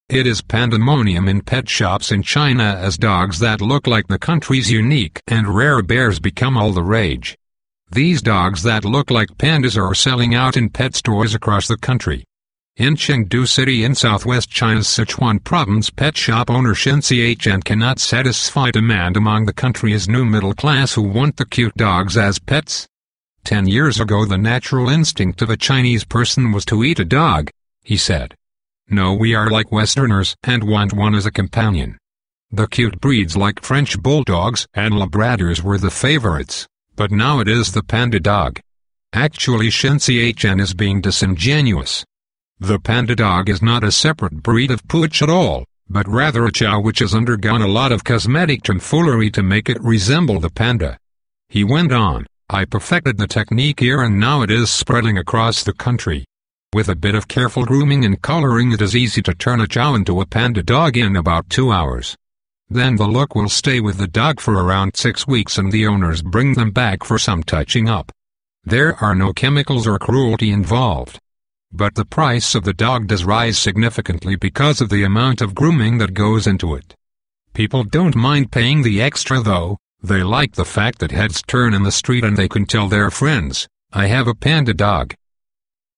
Chow Panda 'Lookalike' Dogs Are Hit With China's Middle Class
It is panda-monium in pet shops in China as dogs that look like the country's unique and rare bears become all the rage. These dogs that look like pandas are selling out in pet stores across the country. In Chengdu city in southwest China's Sichuan province pet shop owner Hsin Ch'en cannot satisfy demand among the country's new middle class who want the cute dogs as pets. 'Ten years ago the natural instinct of a Chinese person was to eat a dog,' he said. 'Now we are like westerners and want one as a companion. The cute breeds like French bulldogs and Labradors were the favourites, but now it is the panda dog.' Actually Hsin Ch'en is being disingenuous. The panda dog is not a separate breed of pooch at all, but rather a chow which has undergone a lot of cosmetic tomfoolery to make it resemble the panda. He went on: 'I perfected the technique here and now it is spreading across the country. 'With a bit of careful grooming and colouring it is easy to turn a chow into a panda dog in about two hours. 'Then the look will stay with the dog for around six weeks and the owners bring them back for some touching up. 'There are no chemicals or cruelty involved. 'But the price of the dog does rise significantly because of the amount of grooming that goes into it. People don't mind paying the extra though - they like the fact that heads turn in the street and they can tell their friends: "I have a panda dog".